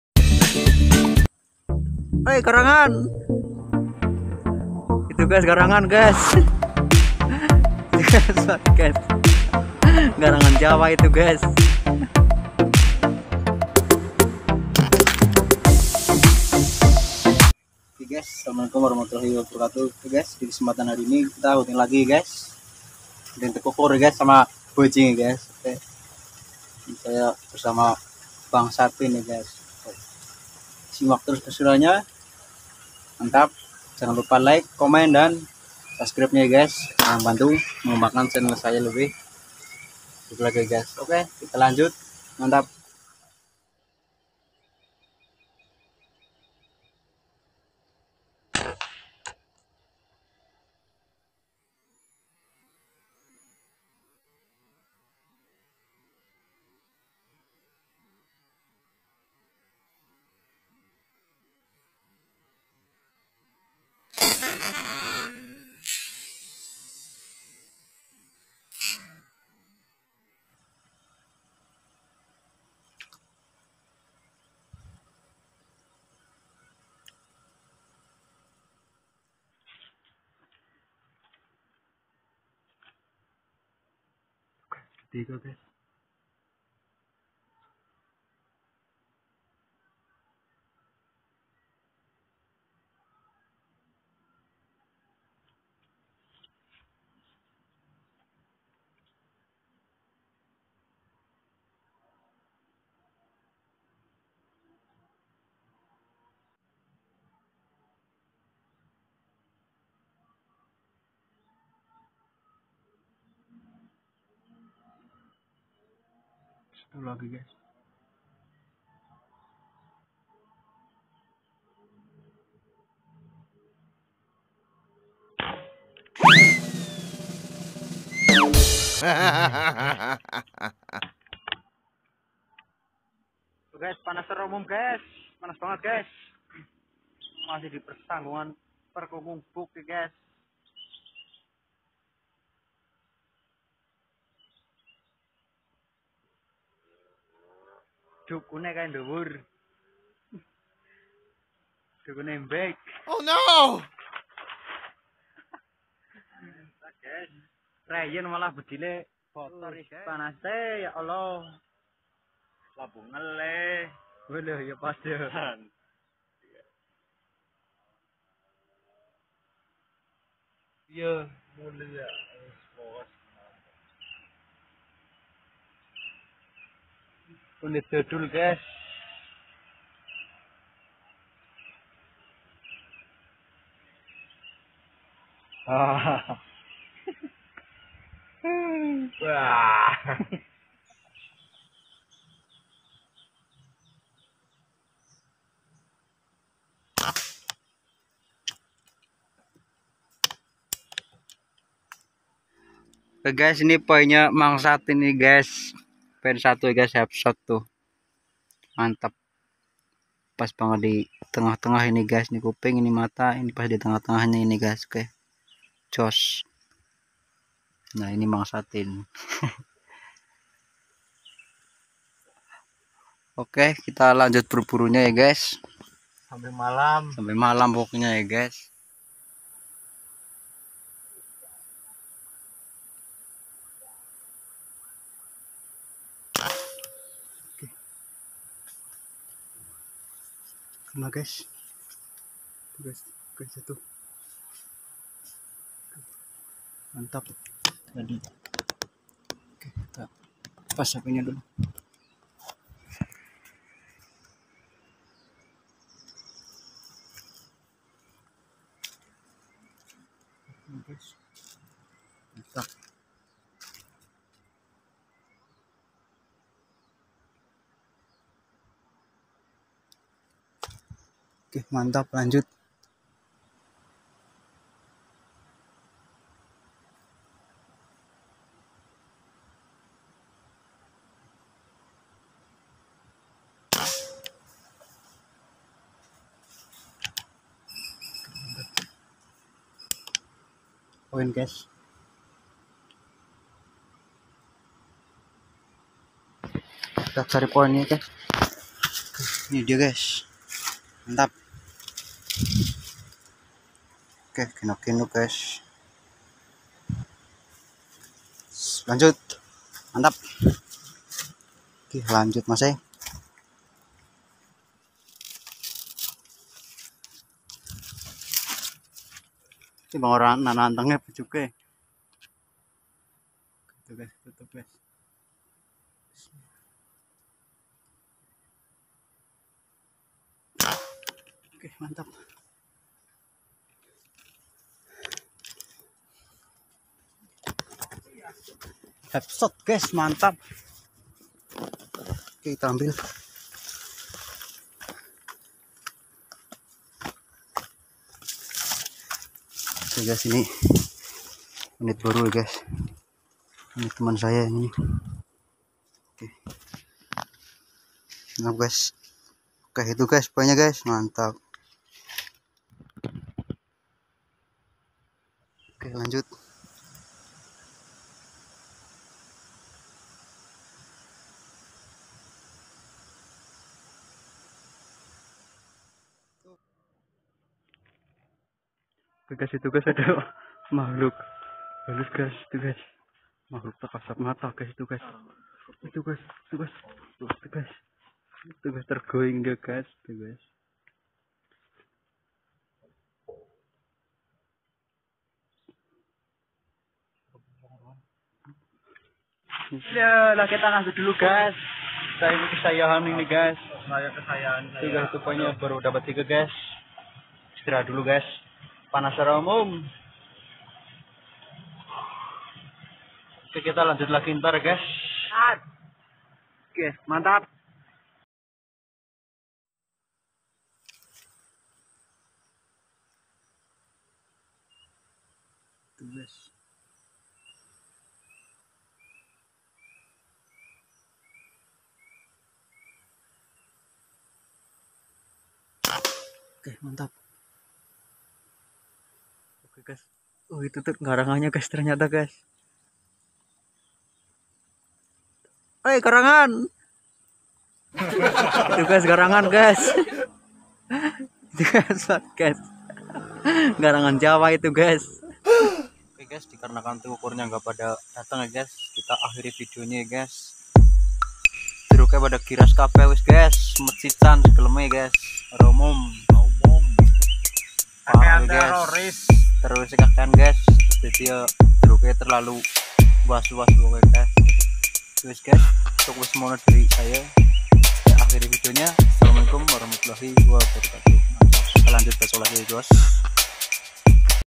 BERBURU TEKUKUR DAN TUPAI
berburu tekukur disambung berburu tupaii cuaca sangat panas banget tapi tidak patah semangat tetap berburu.